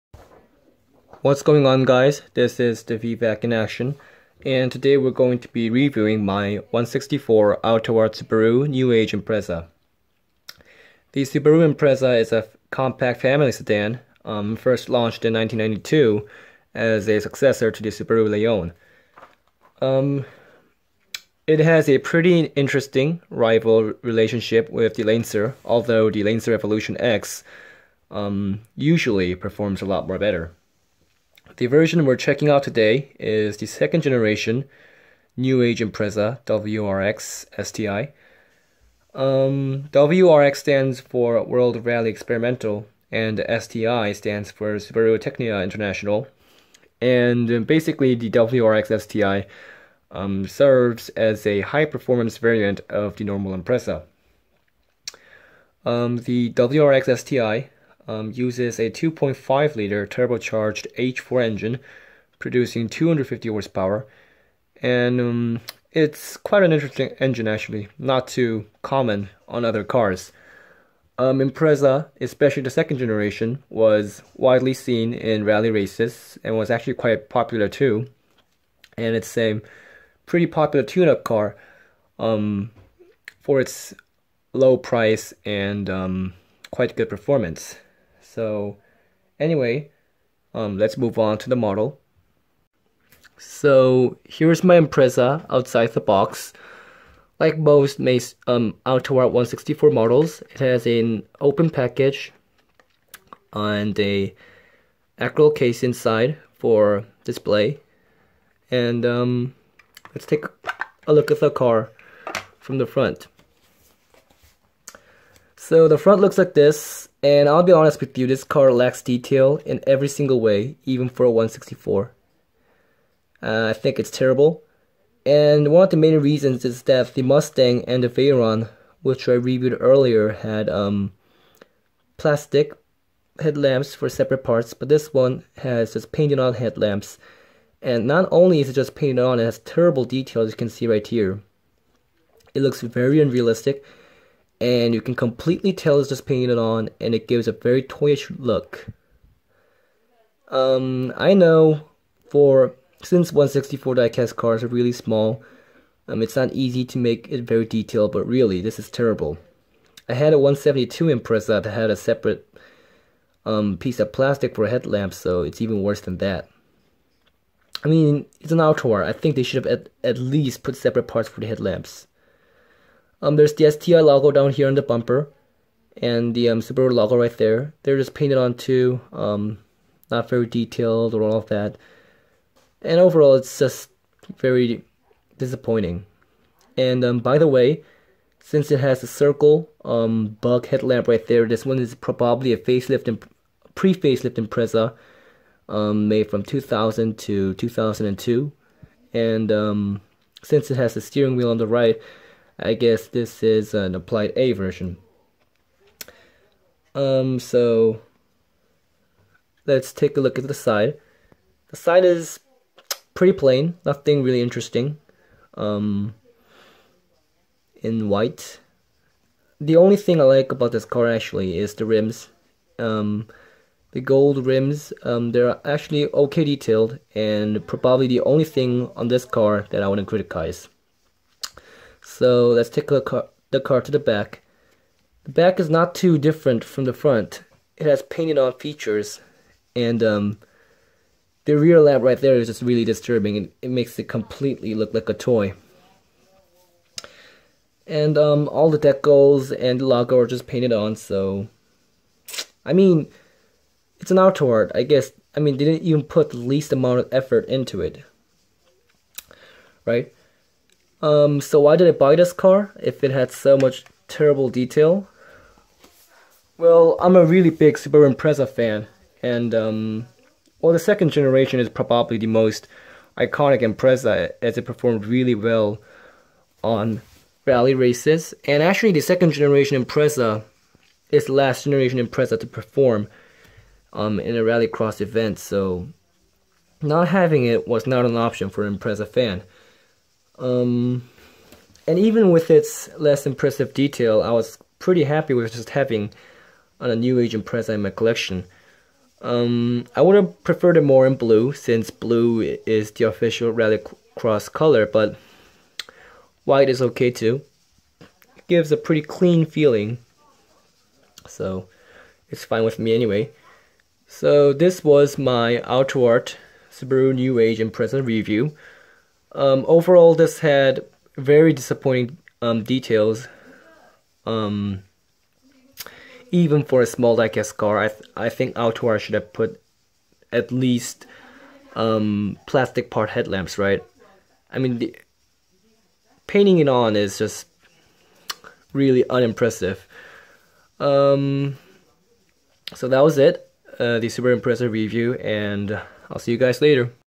Part 3 1/64 Autoart Subaru New Age Impreza WRX Sti Review
-Poor detail in every single way...still it's a Impreza